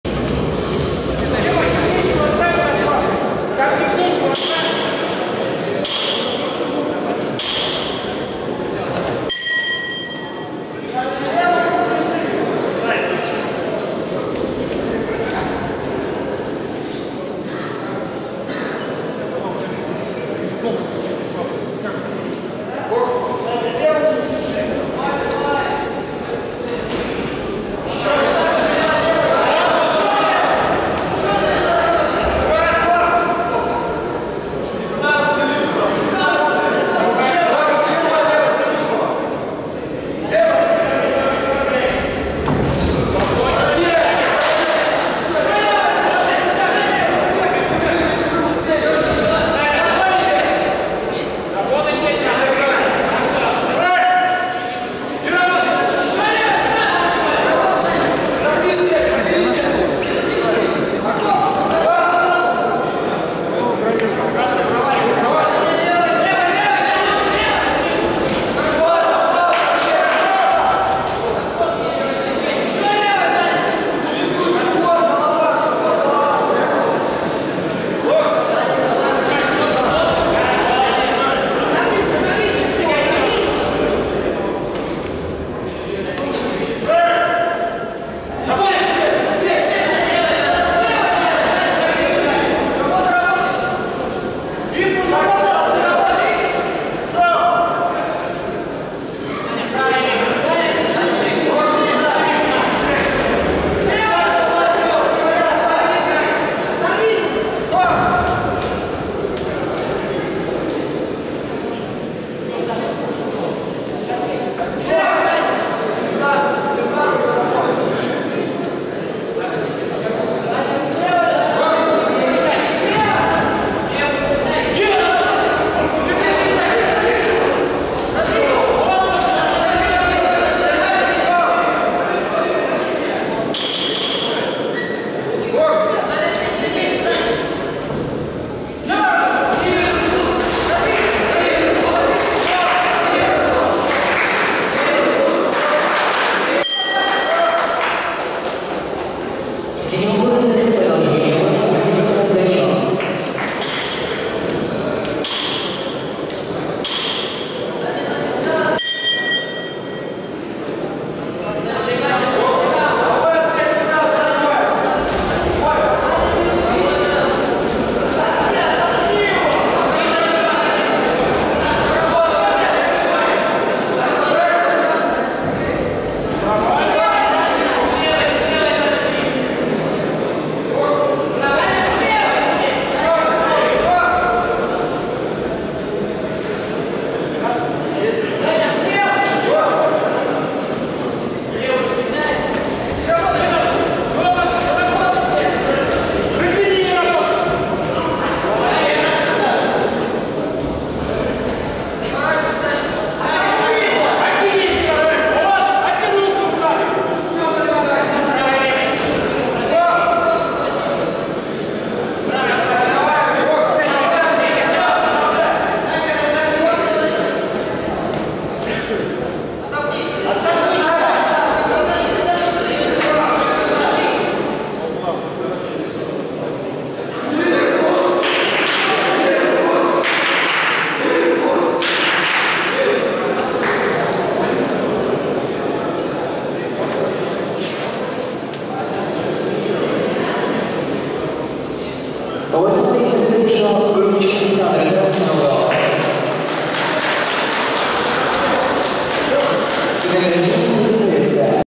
Викторов Даниил (Нижнеудинск) vs Груничев Александр (Томск)
Первенство Сибирского федерального округа по боксу среди юношей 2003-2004гг.р. (г.Яровое, Алтайский кр., 4-9 февраля 2019г.)
ФИНАЛ до 66кг